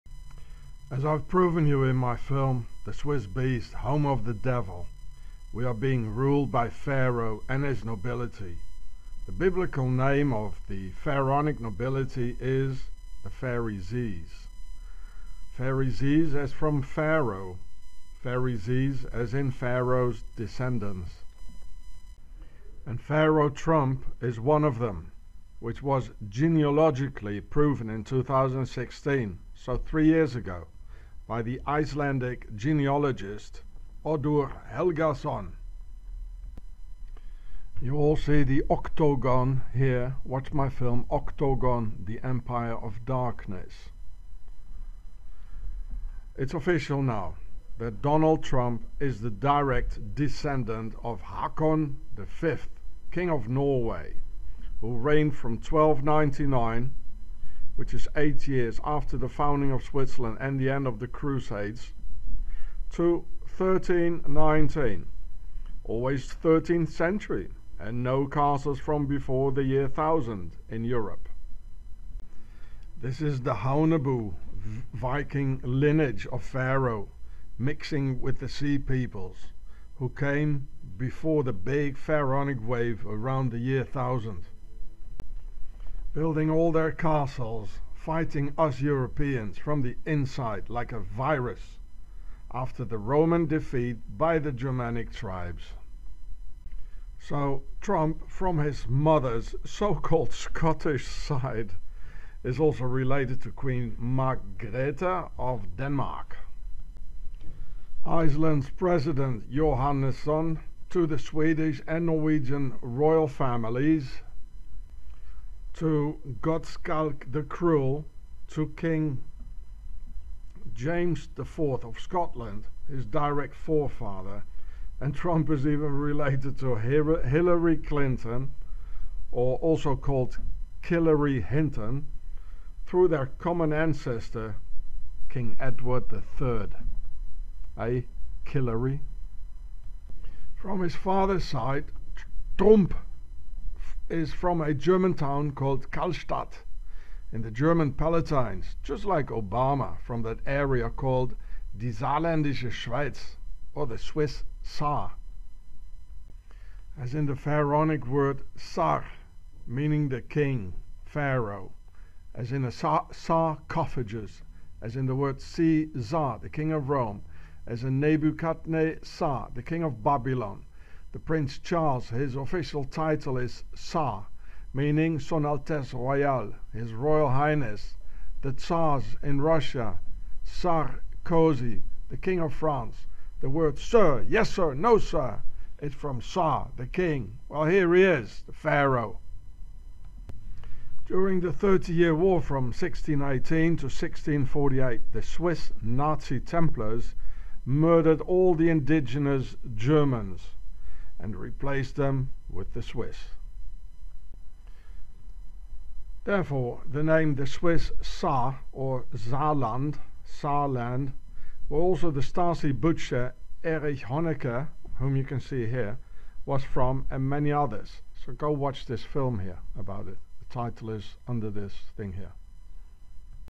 The Real Origins of President Donald Trump Revealed - "it's still a Feudal System, folks"
Humanity is a Slave Race of Pharaoh and their Anunnaki Branch. Total intel in video "The Swiss Beast - Home of the Devil".
GENEALOGICAL EXPERTS HAVE PROVEN KING TRUMP THE 1ST HIS ROYAL TIES OF PHARAOH'S COSMOPOLITAN BLOODLINES.